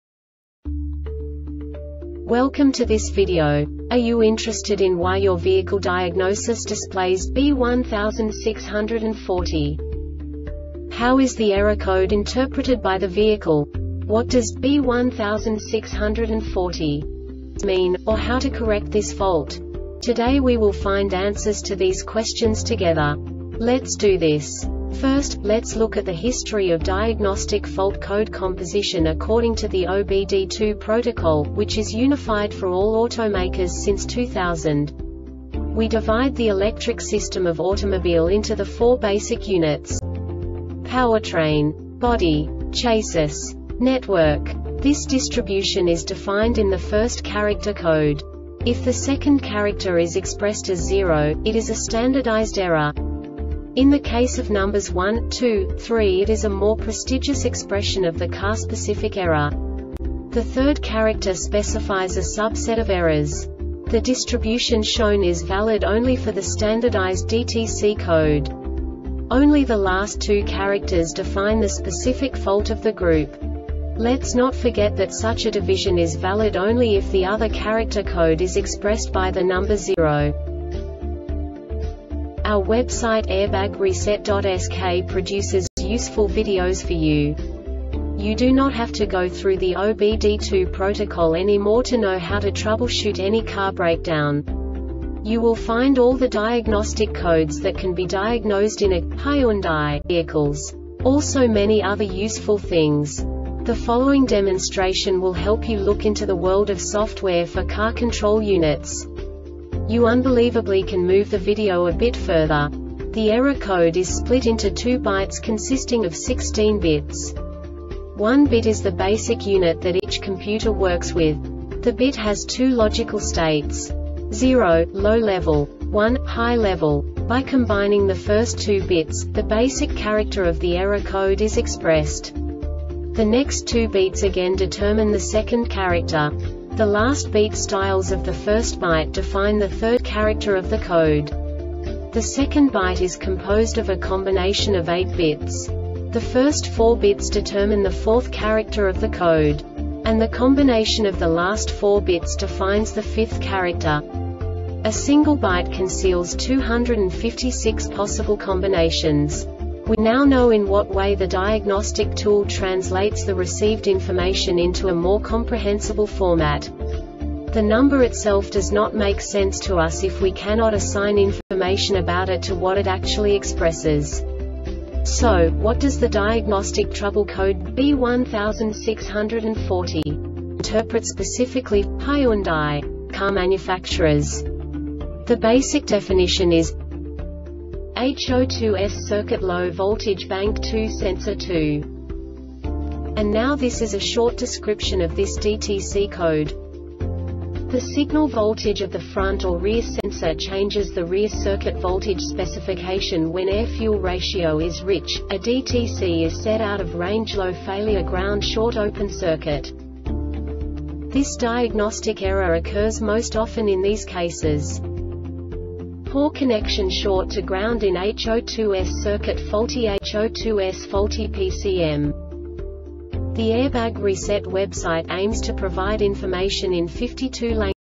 DTC Hyundai B1640 Short Explanation
Contents:
0:21 Basic DTC analysis according to OBD2 protocol standard.
1:48 Insight into programming
2:58 A diagnostically understandable description of the Diagnostic Trouble Code
3:05 Basic error definition

Make:
Hyundai
Code:
B1640
Definition:
HO2S Circuit Low Voltage (Bank 2 Sensor 2)
Description:
The signal voltage of the front or rear sensor changes the rear circuit voltage specification when air fuel ratio is rich,a DTC is set.Out of range low failure (ground short open circuit).
Cause:
 Poor connection Short to ground in HO2S circuitFaulty HO2S Faulty PCM